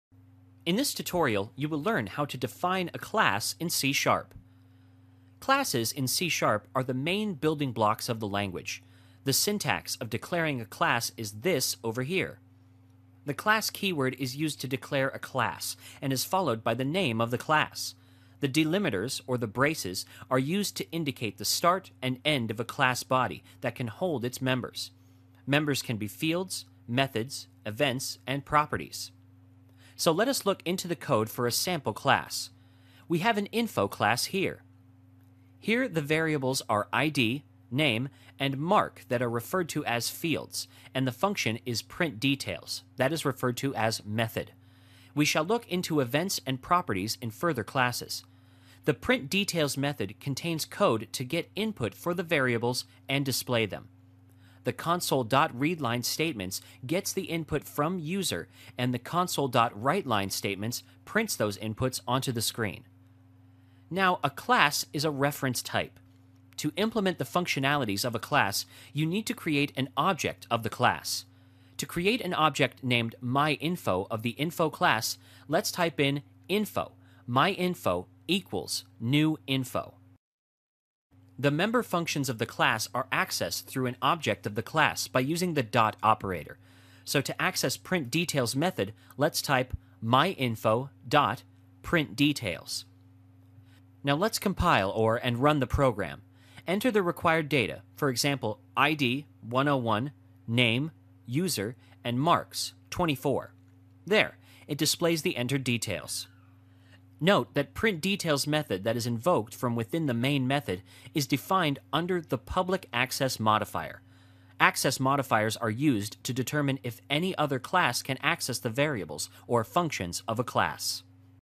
How to Define a Class in C#
This tutorial is about how to define class in C#. Class can be defined as a template for the objects

Introduction

Classes in C# are the main building blocks of the language. The syntax of define a class in c# is as follows:
Class (classname)

The class keyword is used in c to declare a class and is followed by the name of the class. Class can be defined as a template for the objects. The delimiters or the braces are used to indicate the start and end of a class body that can hold its members. Members can be fields, methods, events and properties.

Step 1: Declaring a class
The following code snippet is an example on how to define class in C#:
class Info
    int id;
    string name;
    int mark;

    public void PrintDetails()
       Console.WriteLine("Please Enter your Id \t:");
            id = Convert.ToInt32(Console.ReadLine());
            Console.WriteLine("Please Enter your name \t:");
            name = Console.ReadLine();
            Console.WriteLine("Please Enter your mark \t:");
            mark = Convert.ToInt32(Console.ReadLine());
            Console.WriteLine("Id \t:" + id);
            Console.WriteLine("Name \t:" + name);
            Console.WriteLine("Mark \t:" + mark);
Here the variables are id, name and mark that are referred to as fields and the function is PrintDetails() that is referred to as method. Fields are the data belonging to the class and methods define the behavior of the class.

Step 2: Initializing a class
A class is a reference type. To implement the functionalities of a class, you need to create an object of the class. We have used the preceding code snippet of C, to declare a class named info. To create an object named myInfo of the info class, the following code snippet is given in the Main() method:
Info myInfo = new Info();

Step 3: Accessing a class member
The member functions of the class are accessed through an object of the class by using the dot operator, as shown in the following code snippet:
myInfo.PrintDetails();

Here the PrintDetails() method is invoked by the myInfo object of the class Info. This function gets the id, name and mark from the user and displays the same, respectively.

Conclusion
A collection of classes is called a namespace.  C# provides certain predefined set of classes and methods. The member function that is invoked from within the Main() method is defined under the public access modifier. Access modifiers are used to determine if any other class can access the variables or functions of a class. You will learn more about them in the subsequent chapters.